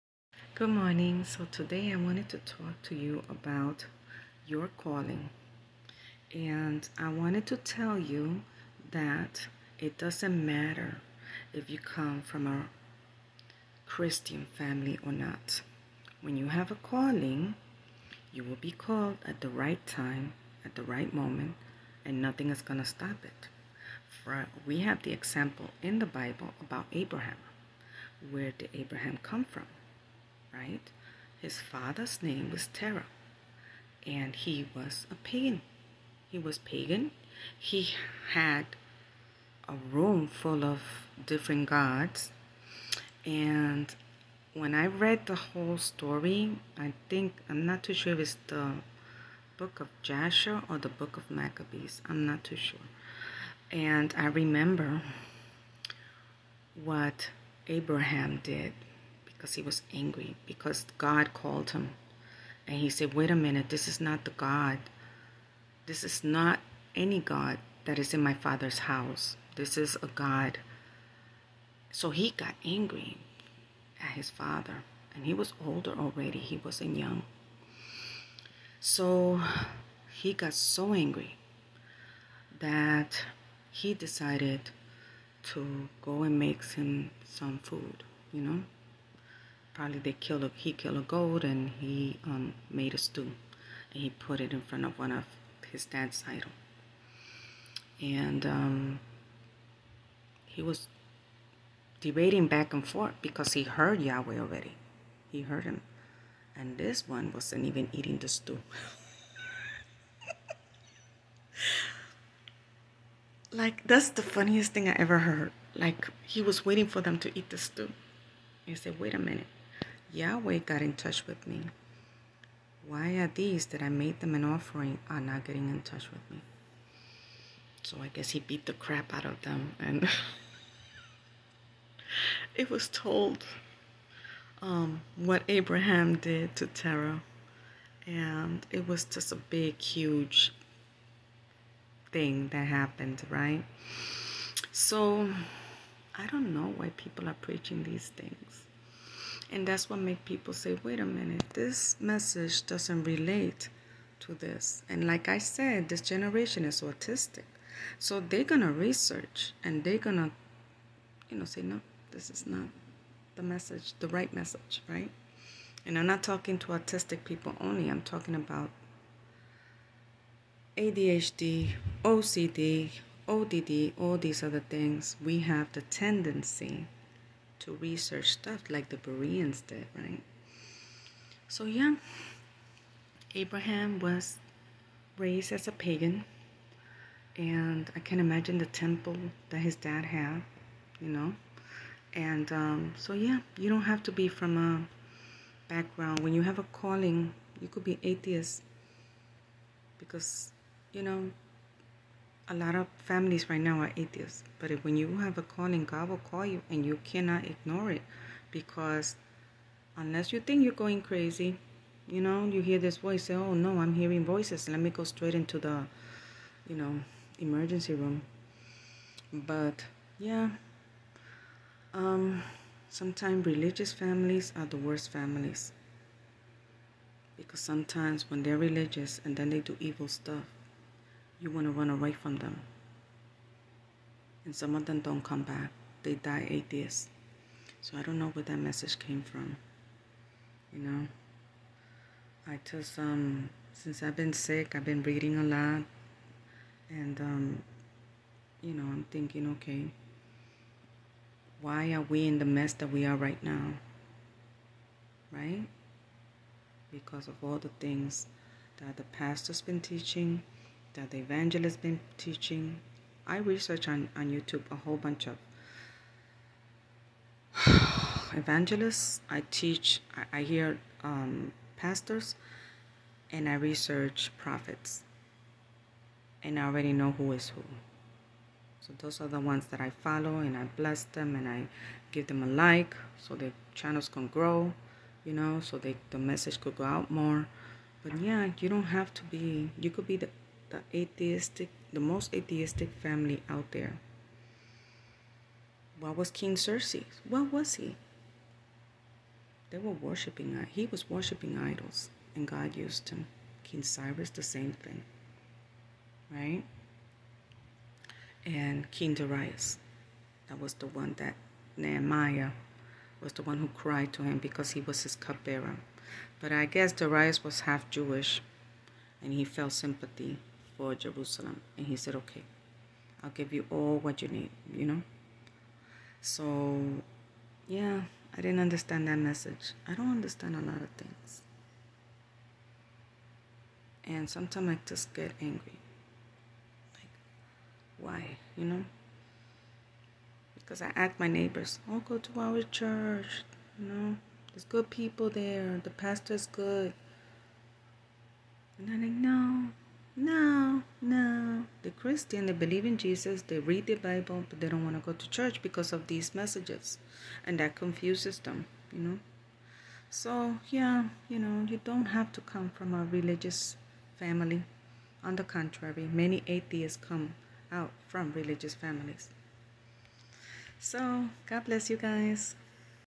Our father Abraham and his pagan family
you have to come out of a religious family to get a call from God